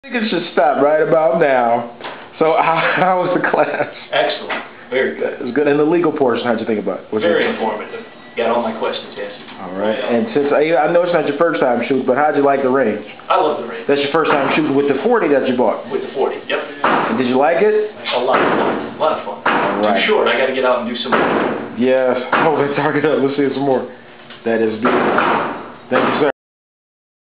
Detroit Michigan CCW Class Student Video Testimonial #75
- A student shares his thoughts about our Personal Protection Class.